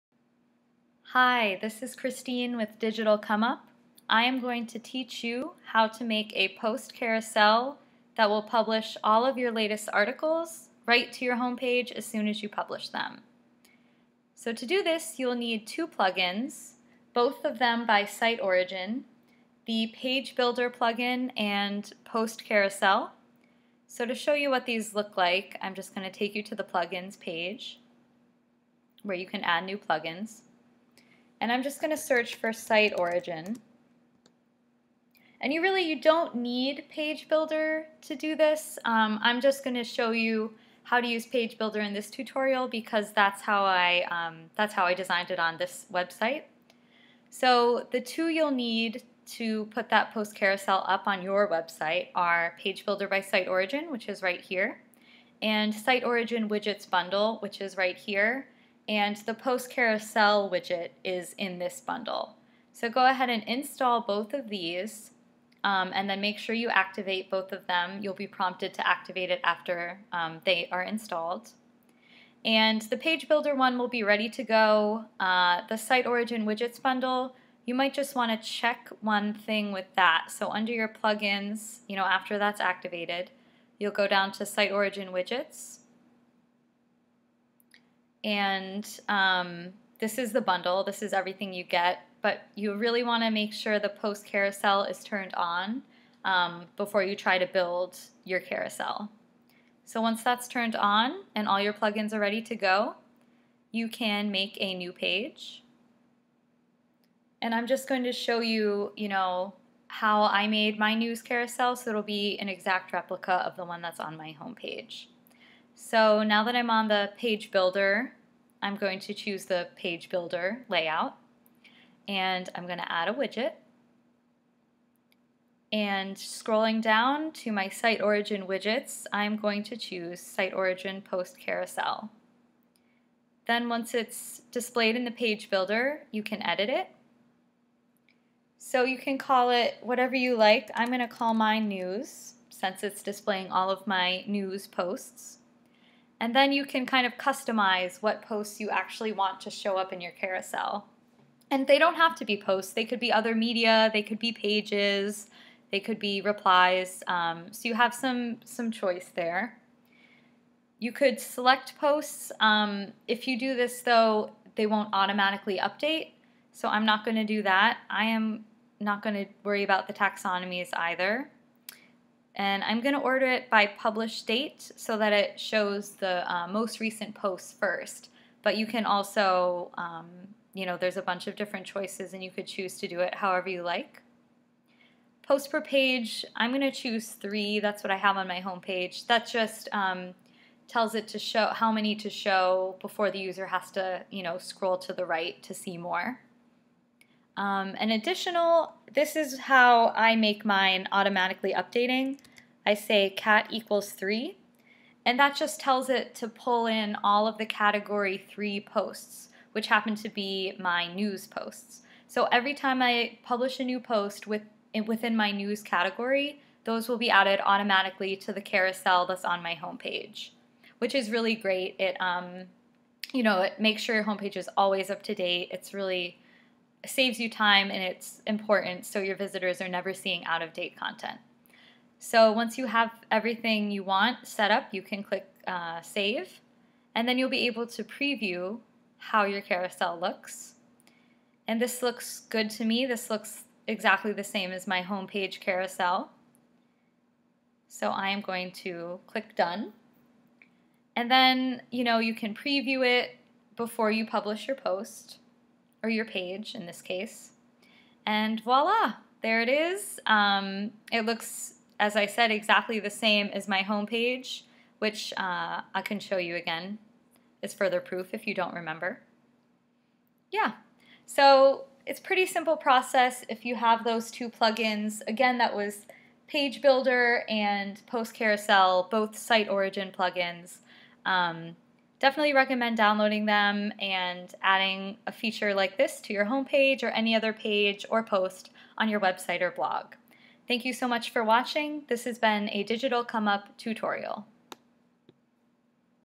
How to: Create a Self-Updating Post Carousel
In this Digital Come Up tutorial, you will learn how to create a responsive, attractive, and interactive carousel that displays your latest content on your homepage or any other page of your website or blog. For more WordPress tips and tricks to help you with web design,